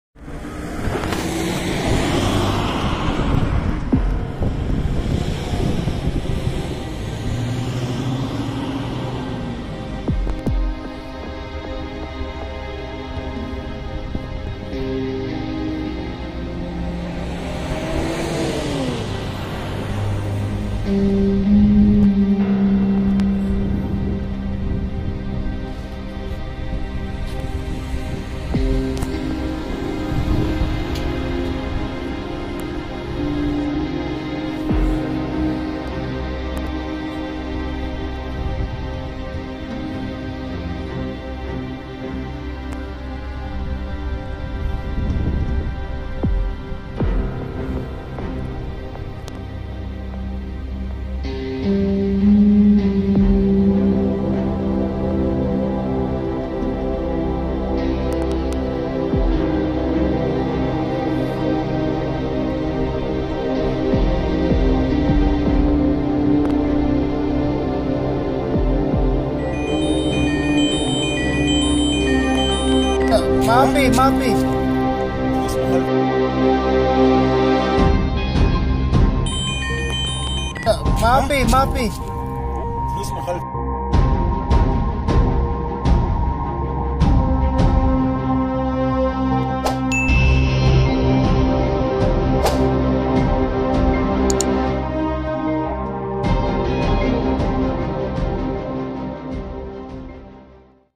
What Would You Do If A Dragonfly Turns to Dragon?
This is a suspense situation when I was waiting outside the Hospital in Saudi Arabia. I noticed a dragofly flying towards me and landed besides the gutter.

Very raw video taken on February 2,2020